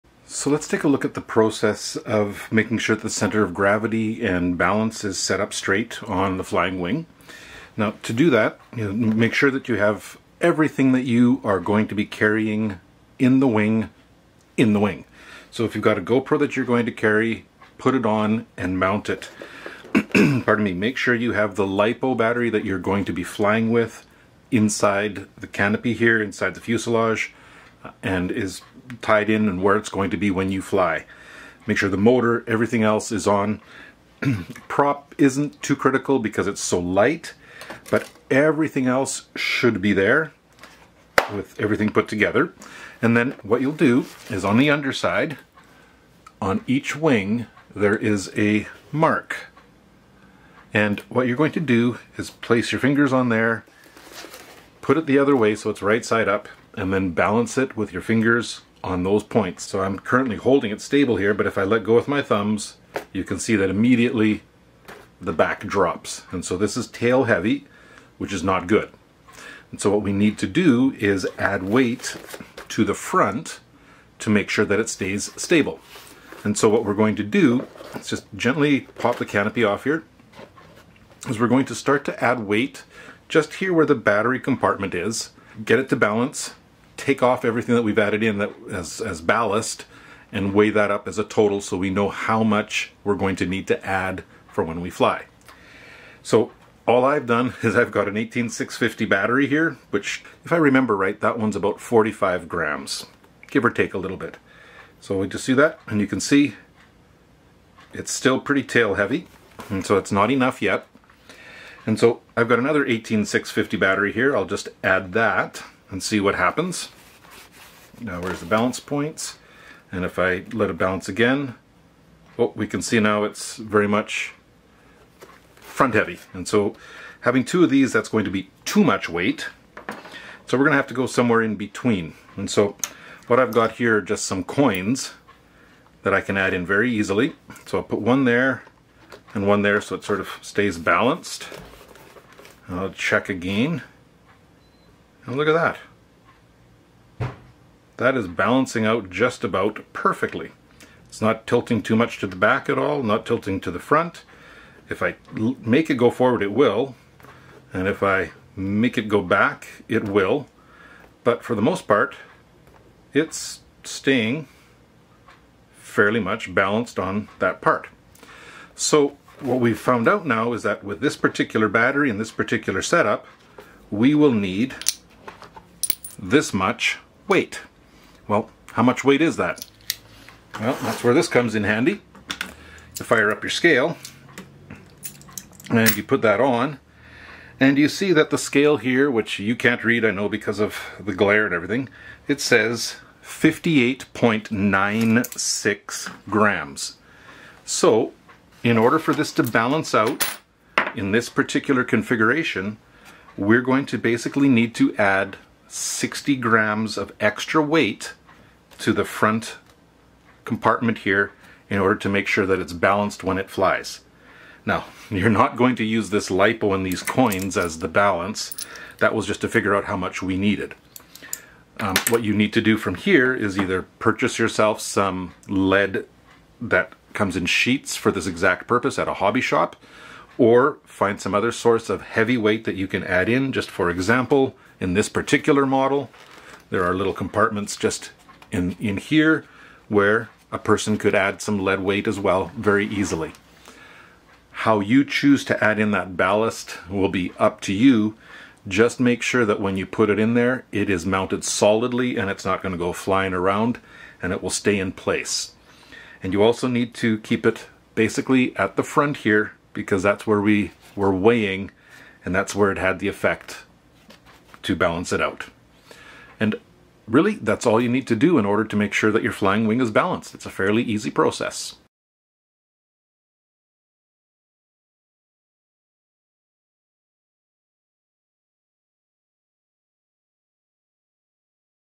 How to Balance a Flying Wing on its CG (center of gravity)
Whenever you fly a fixed wing model, you need to make sure that it is balanced over its center of gravity.  A completely unbalanced flying wing can be impossible to fly, so it needs to be done correctly.  This video will show you how to balance an AR Wing Pro over its CG, but the process is the same for any flying wing.  After you properly balance a flying wing on its CG it will give you a much more stable and manageable flight.